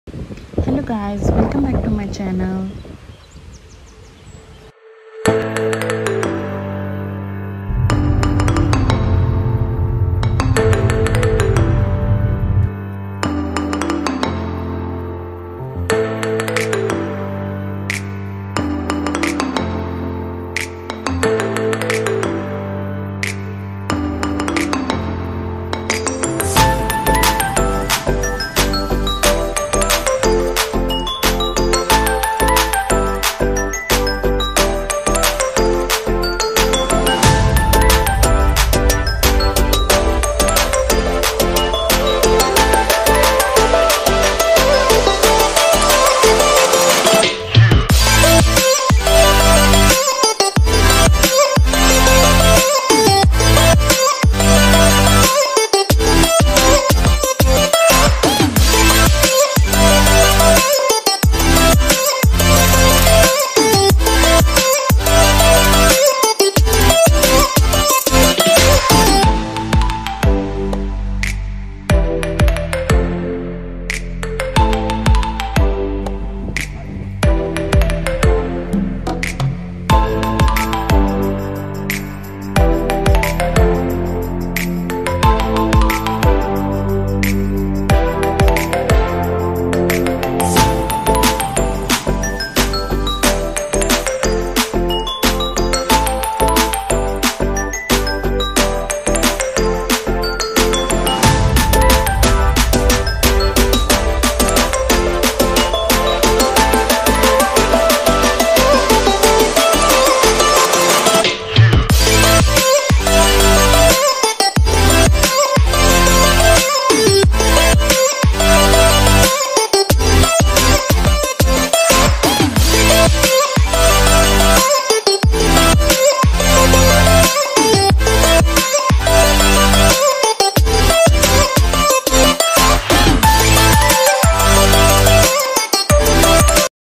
Peterborough Ontario Canada | Summer Vibes | SayRish
Canada Park Tour | Tim Hortons Coffee | Morning Walk
Sandy Lake Beach | Beach in Buckhorn, Ontario